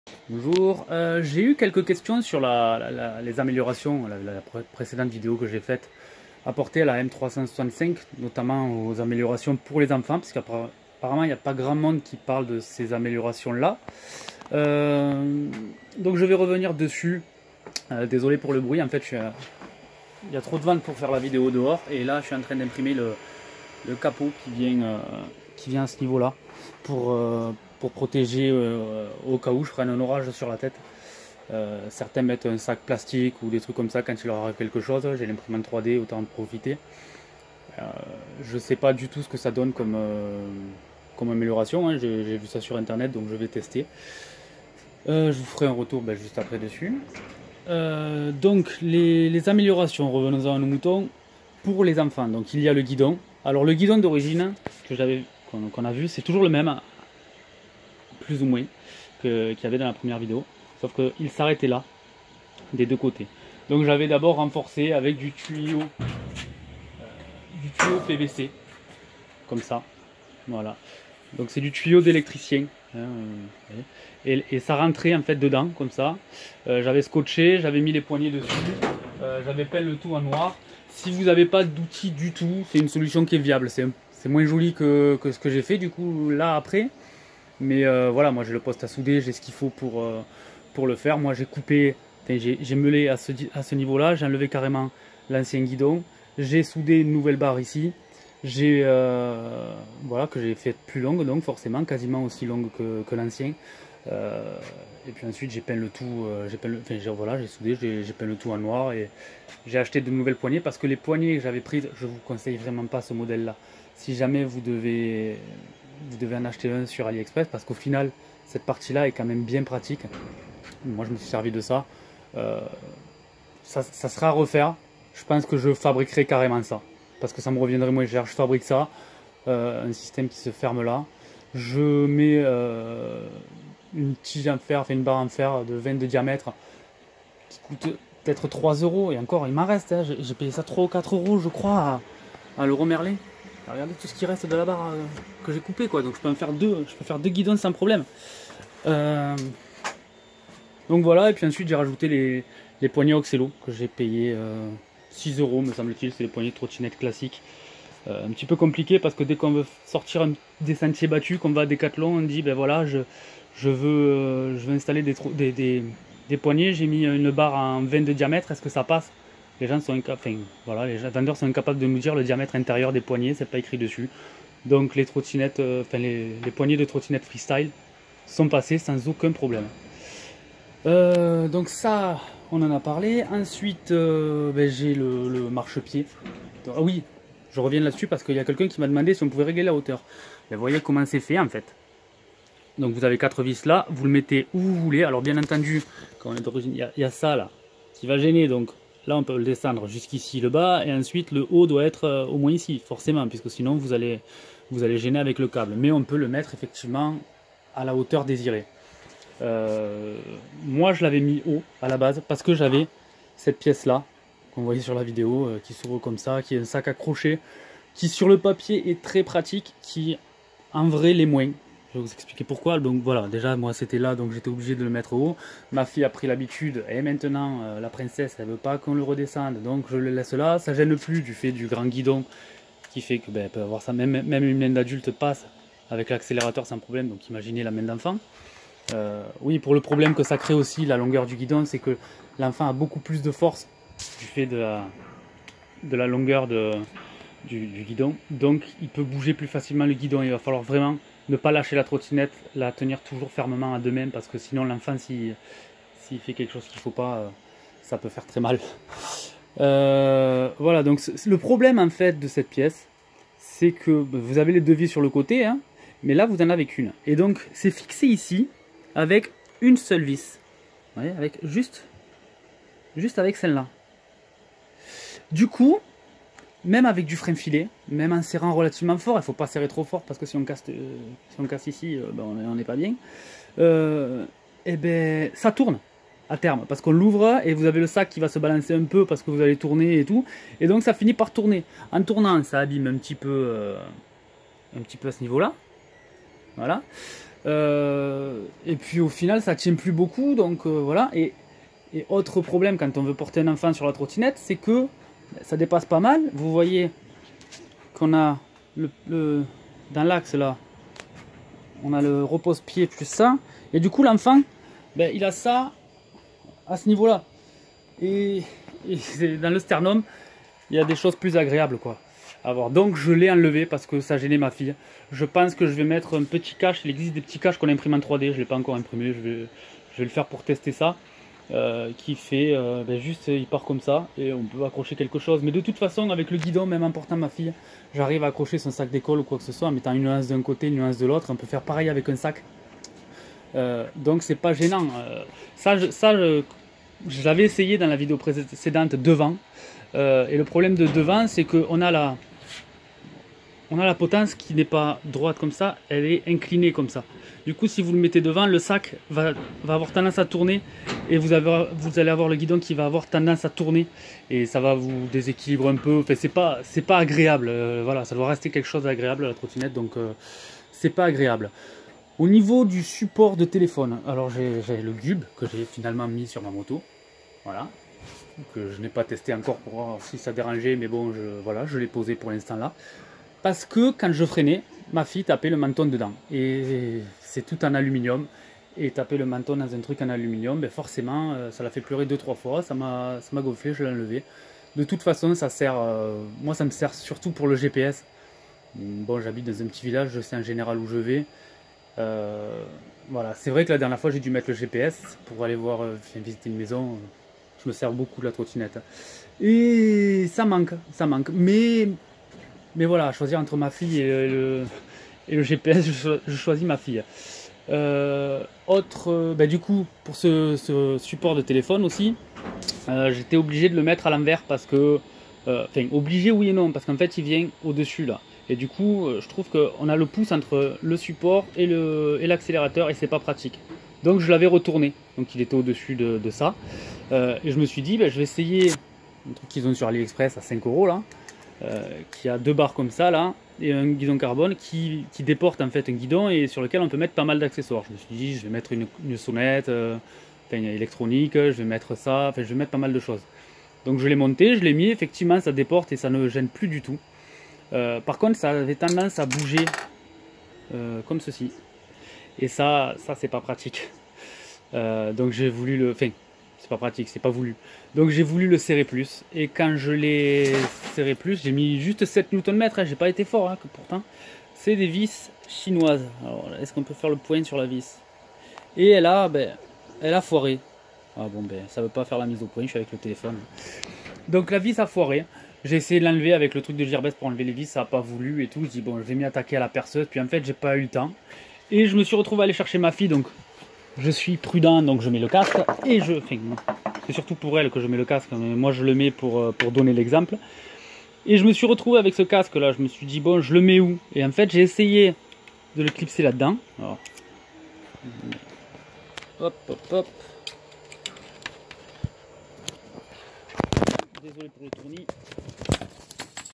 Retour sur les améliorations apportées à ma m365(1)
Suite a ma précédente vidéo,  je fais un retour sur les amélioration apportée a ma m365. Partie 1/2